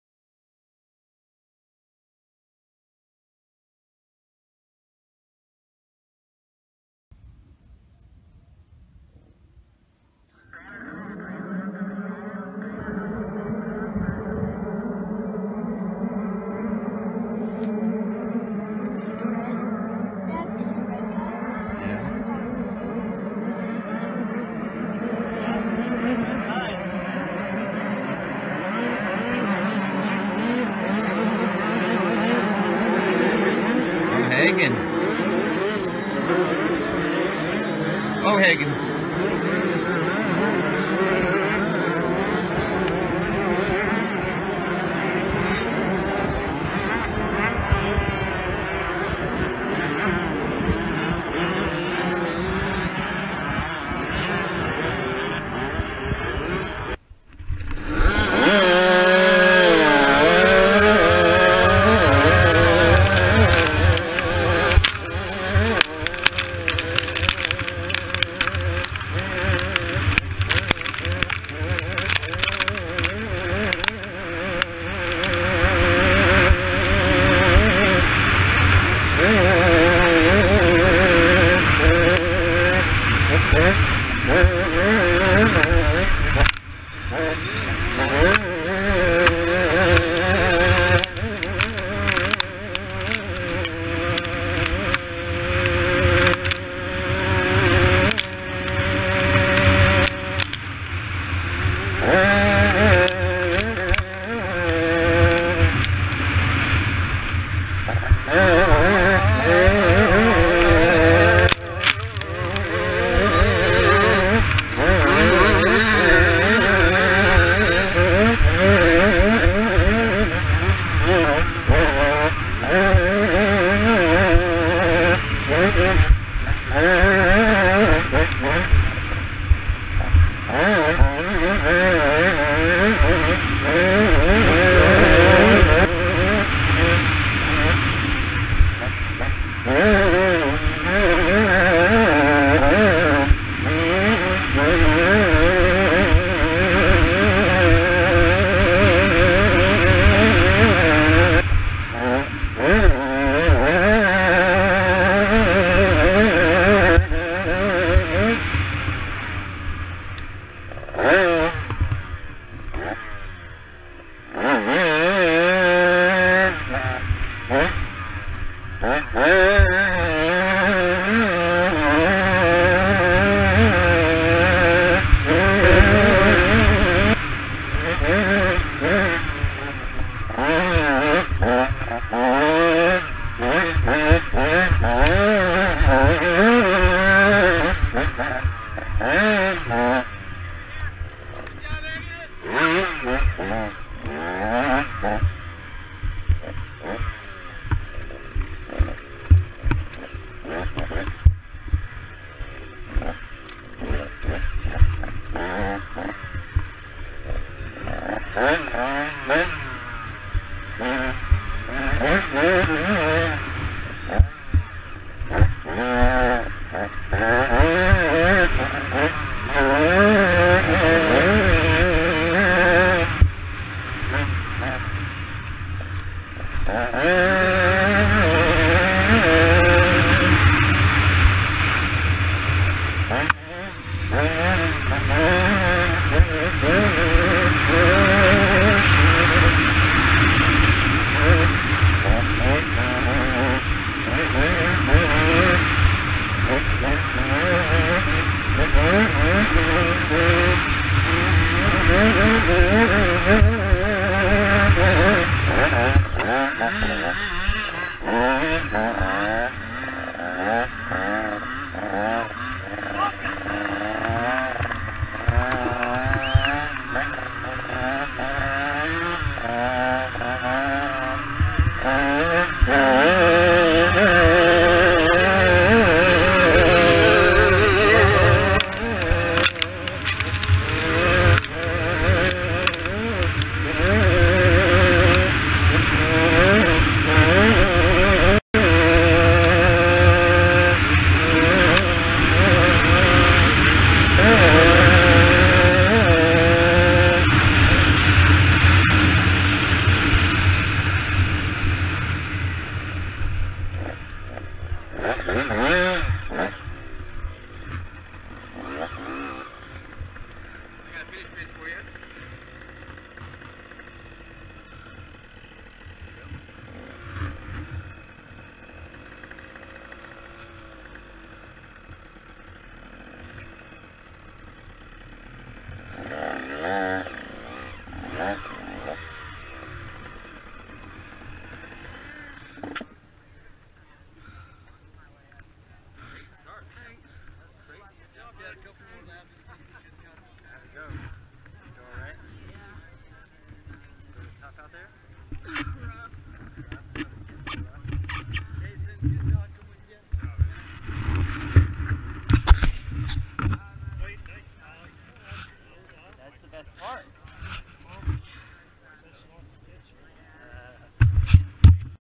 National Youth Hare & Hound Round 6 2016
Short clip of the big wheel supermini race, opening video of bomb run is from a cell phone. Hagen racing in round 6 of the National Youth Hare & Hound series in Red Mountain California. He lead for most of the first lap till before the pits where he was passed by Tyler Lynn who was 1st overall and then passed again on the on the 4th lap coming out of the pits by Mason Matthies who picked up 2nd overall and he held that till the end finishing 3rd overall after 9 laps.